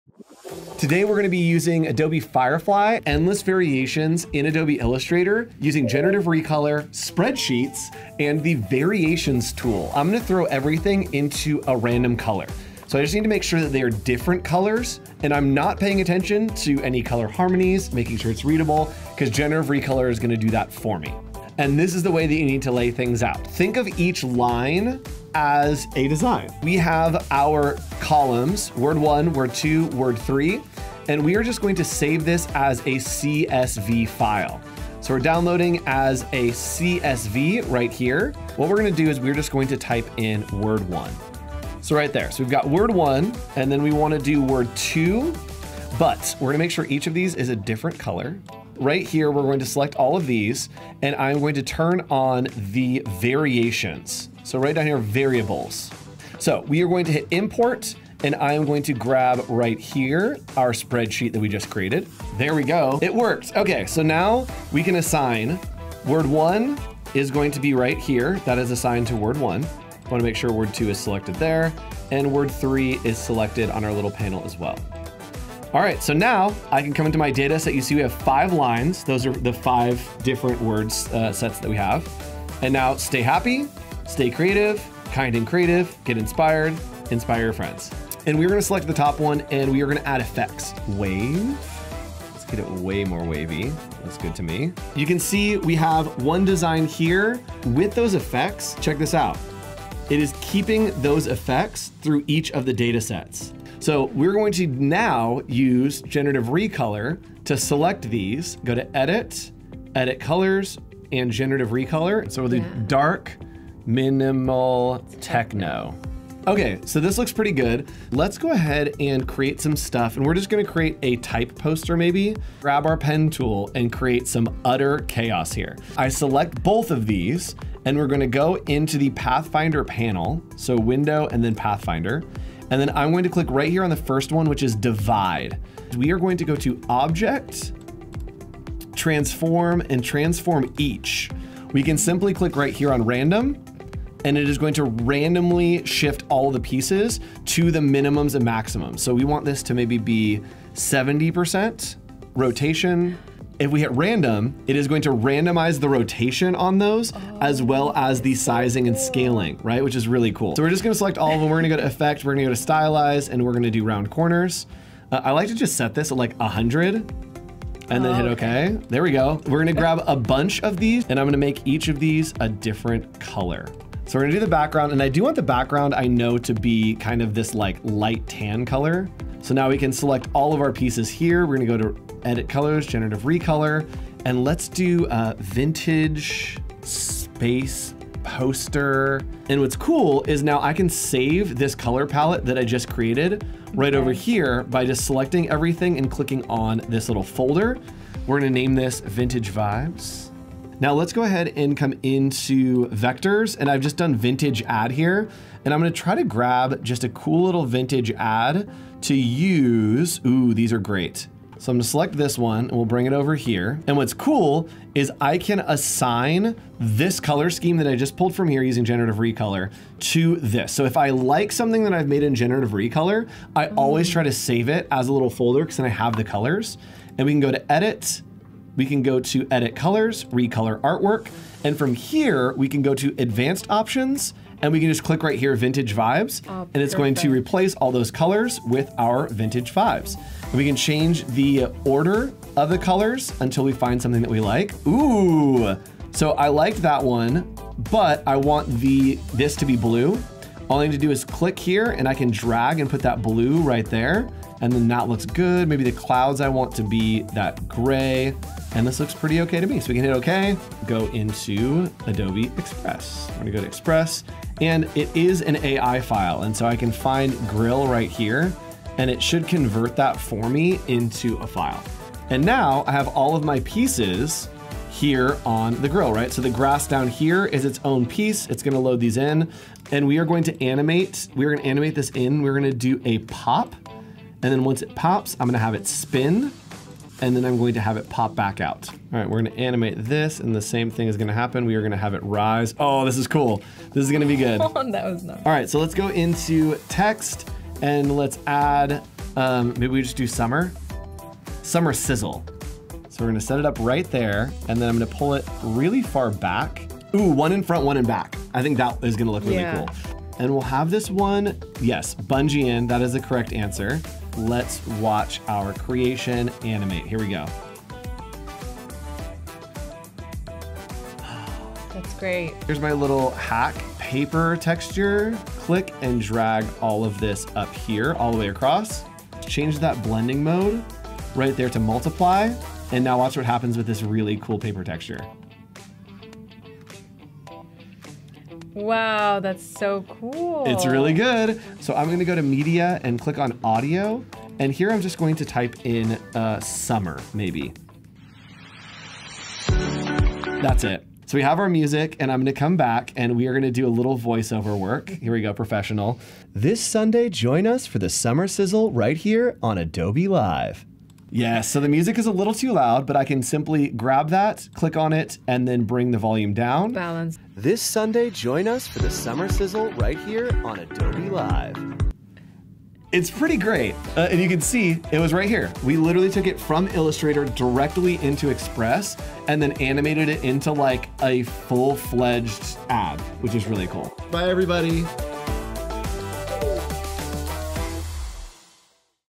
Utilize Generative Color to SPICE up your Color Palettes in Illustrator
Generative Recolor is HERE in Illustrator. Andrew Hochradel and Cyn Lagos show examples of how to effectively use this new tool in your workflow! Plus hear from these two pros on how they choose color variations for their projects.